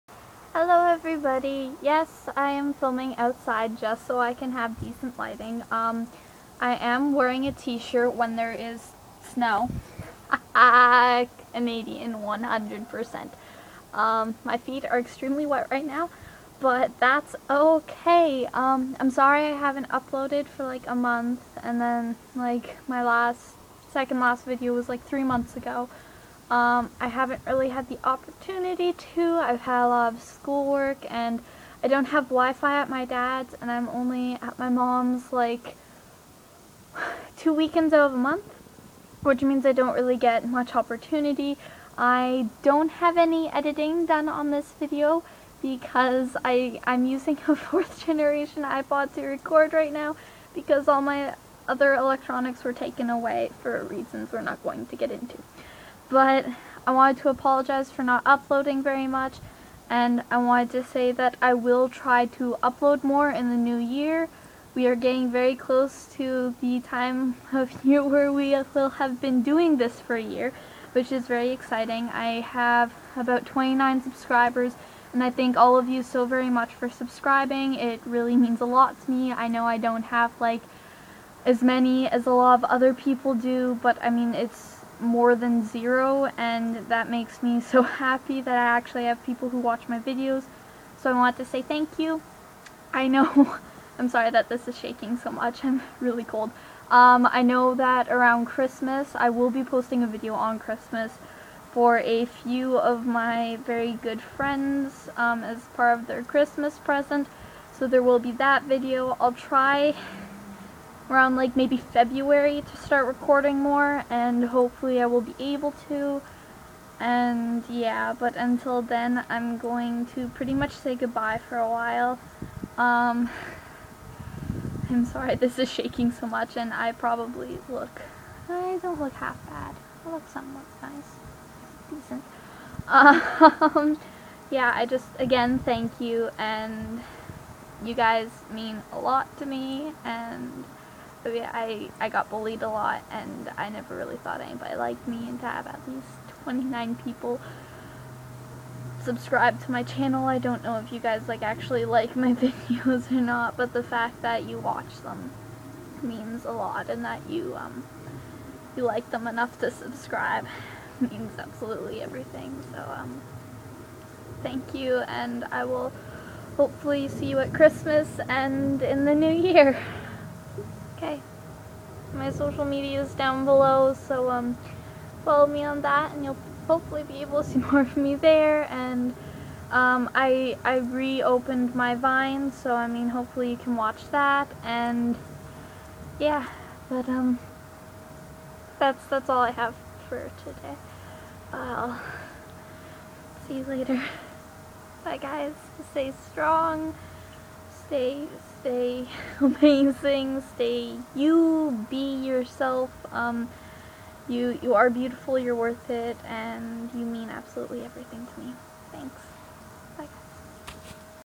Saying goodbye for now...
It hopefully won't be for long and I hopefully will get better filming equipment at Christmas or something but until then goodby my 29 subscribers each and everyone of you are extremely special to me and I will hopefully see you at Christmas and in the new year. (: